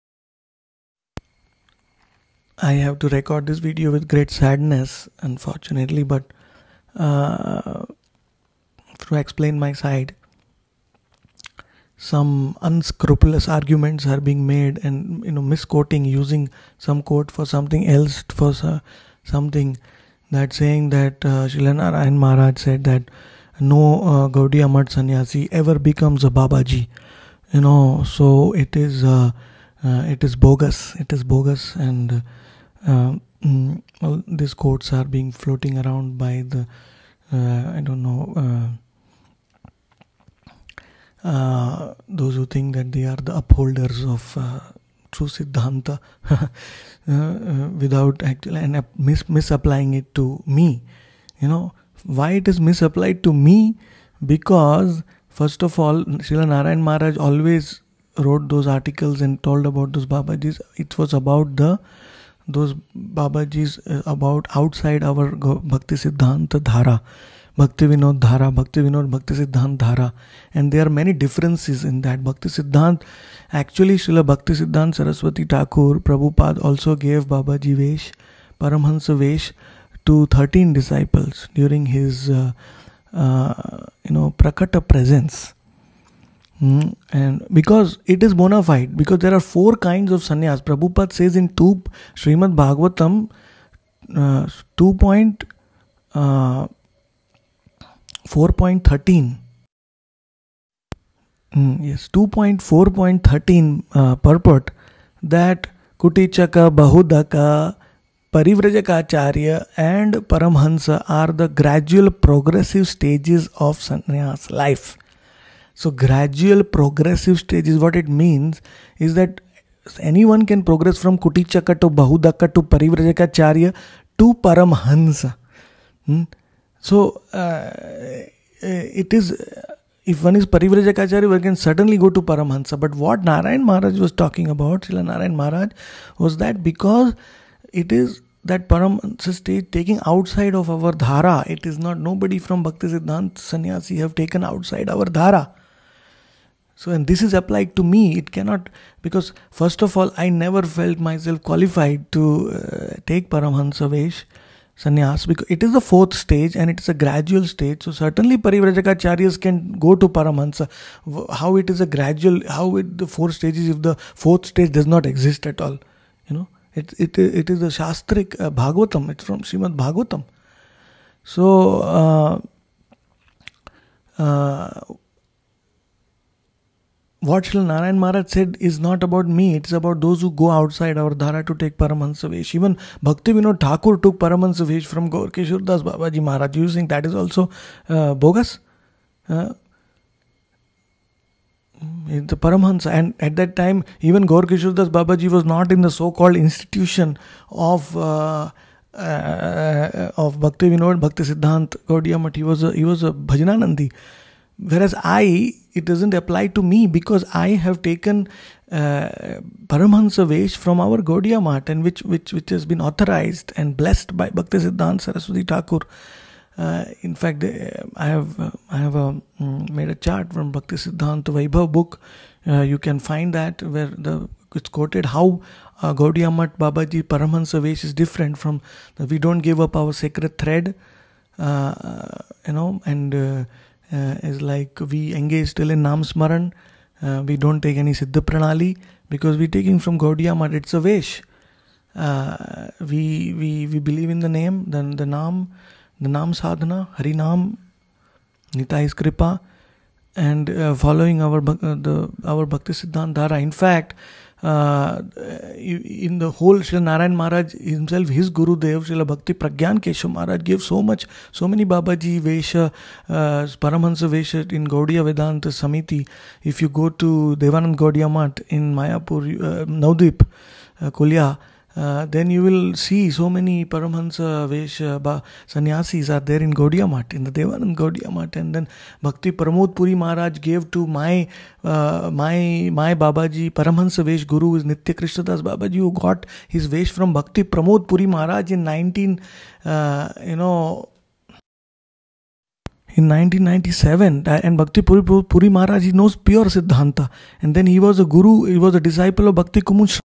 My Paramahansa Vesha in Gaudiya Matha
I have to record this video with great sadness unfortunately but to explain my side some unscrupulous arguments are made and misquoting using some quote for something else saying Srila Narayan maharaj said that no gaudiya matha sanyasi ever becomes a babaji. You know so it is bogus and all these quotes are being floating around by the I dont know. Those who think that they are upholders of true sidhanta and misapplying it to me. Why it is misapplied to me? Because first of all Srila Narayan maharaj always wrote those articles and told about those babajis it was about those babajis outside our Bhaktivinoda Bhaktisiddhant dhara and there are many differences in that. Actually Srila Bhaktisiddhant Saraswati Thakur Prabhupada also gave babaji vesh paramhansa vesh to 13 disciples during his prakat presence because it is bonafide. There are 4 kinds of sanyas. Prabhupad says SB 2.4.13 purport Kuṭīcaka, bahūdaka, parivrājakācārya and paramahaṁsa are the gradual progressive stages of sanyas life. So gradual progressive stages what it means is that anyone can progress from Kuṭīcaka to bahūdaka to parivrājakācārya to paramahaṁsa. So if one is parivrājakācārya, one can suddenly go to paramhansa but what Srila Narayan maharaj was talking about is that because it is paramhansa stage taking outside of our dhara, nobody from Bhaktisiddhant has taken outside our dhara so this is applied to me. First of all I never felt myself qualified to take paramhansa vesha sanyas. It is 4th stage and it is a gradual stage. So certainly parivrājakācārya can go to paramhansa. How is it 4 stages if 4th stage doesnot exist at all? It is shastric, it is from Srimad Bhagvatam.

So what Srila Narayan maharaj said is not about me but it is about those who go outside our dhara to take paramhansa vesha. Even Bhaktivinoda Thakur took paramhansa vesh from Gaura Kishor das babaji maharaj.  Do you think that is also bogus? He is the paramhansa. And at that time Srila Gaurkishor das babaji was not in the so called institution of Bhaktivinoda Bhaktisiddhant gaudiya math. He was a bhajnanandi. Whereas it doesnot apply to me because I have taken paramhansa vesha from our gaudiya matha which has been authorized and blessed by Bhaktisiddhant Saraswati Thakur.

Infact I have made a chart from Bhaktisiddhanta vaibhav book, you can find that which quoted how gaudiya math babaji paramhansa vesha is different. We dont give up our sacred thread and we engage still in nam smaran, we dont take any siddha pranali because we are taking from gaudiya math it's a vesha, we believe in the name, the nam sadhana, Harinam, Nitai's kripa and following our Bhaktisiddhant dhara. Infact in the whole Srila Narayan Maharaj himself his own Gurudev Bhakti Pragyan Keshav maharaj gave so many babaji Paramhansa vesha in gaudiya vedant samiti. If you go to Devananda gaudiya math in Navdvipa kuliya then you can see so many paramhansa vesha sanyasis are there in Devananda gaudiya matha and then Bhaktipramod Puri maharaj gave to my babaji paramhansa vesh guru is Nitya Krishna das who got his vesh from Bhakti Pramod Puri maharaj is 1997 and Bhakti Pramod Puri maharaj knows pure sidhanta.